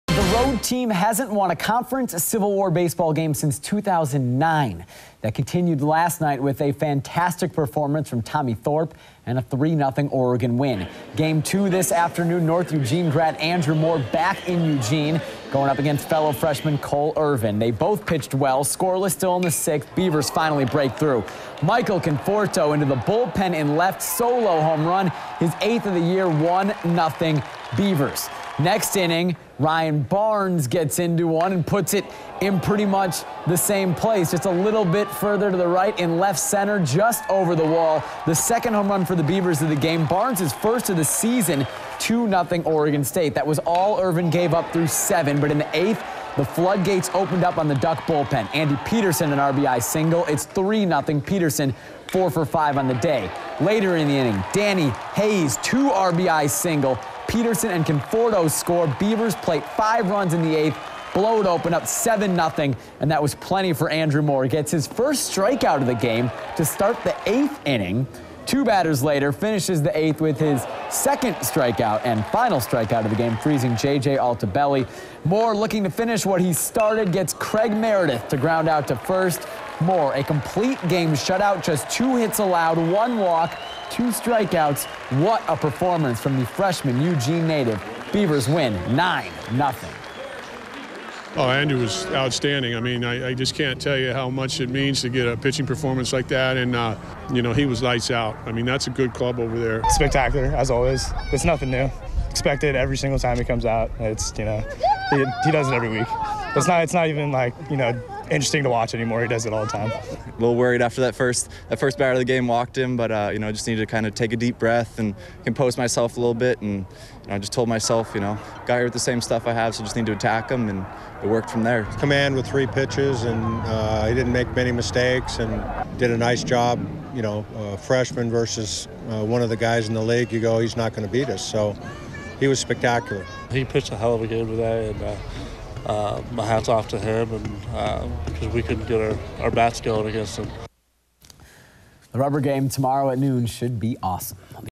2013 Civil War Baseball Series - Game 2 Highlights
Andrew Moore tossed a two-hit shutout and Michael Conforto and Ryan Barnes each hit solo home runs to send the sixth-ranked Oregon State baseball team to a 9-0 win over No. 10 Oregon Saturday afternoon at PK Park.